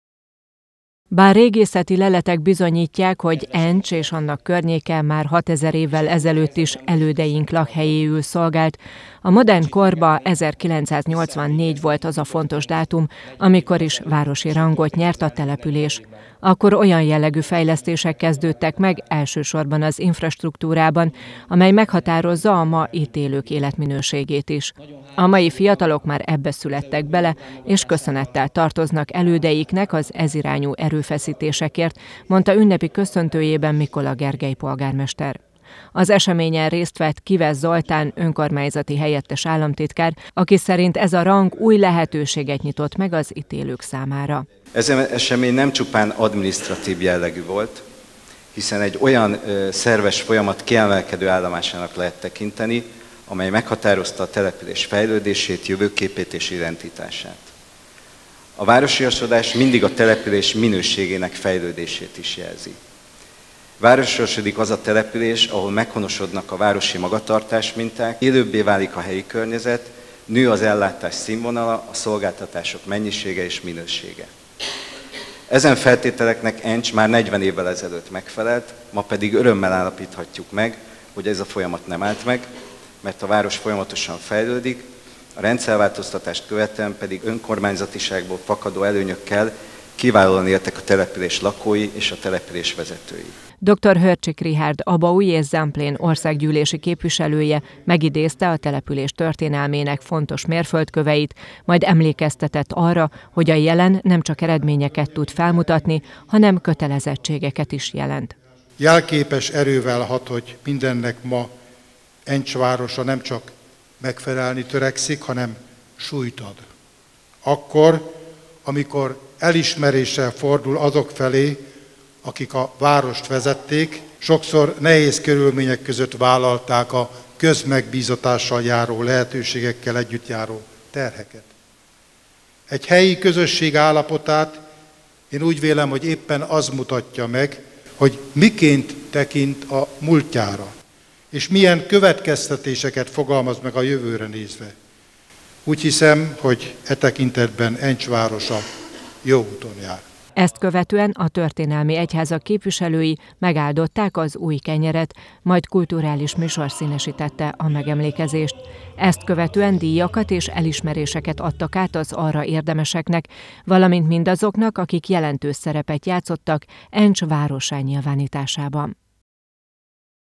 Várossá nyilvánításának 40. évfordulója alkalmából ünnepségre várták az encsiket
40 évvel ezelőtt nyert városi rangot Encs. Ennek jegyében szervezi idei programsorozatát a település. Ezzel együtt államalapító Szent István király ünnepének jegyében előbb a Váci Mihály Gimnázium aulájában szervezett díszünnepségre várták az ott élőket, majd már színes szórakoztató programokkal folytatódott az esemény.